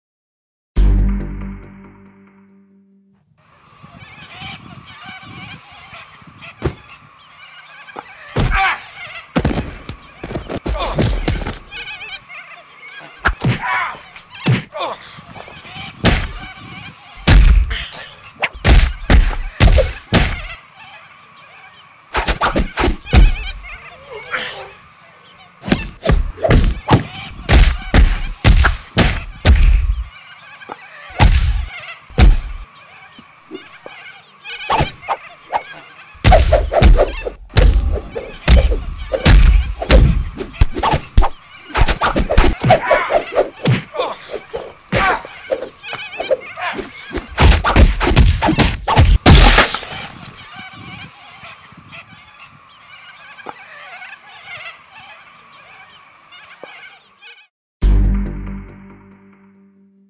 BOXING KANGAROOS!!
You see some interesting stuff when you are out in the Aussie bush looking for thylacines!